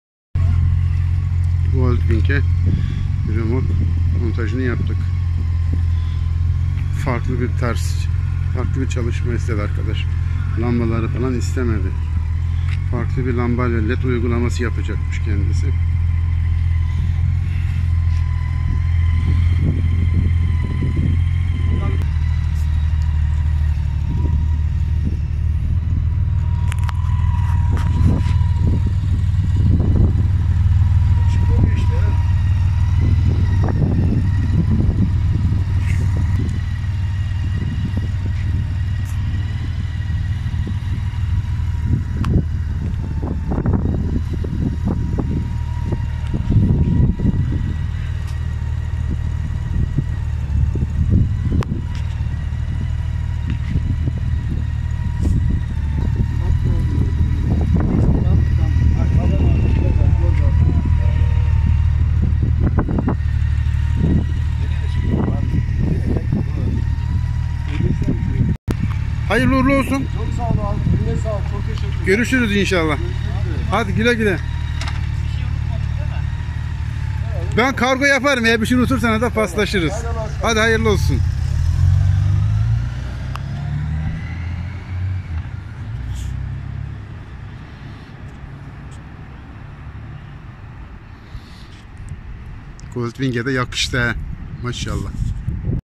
Motorsiklet römorku 532 645 34 77
2014 Goldwing e özel tasarladığım ve ürettiğim tekteker römork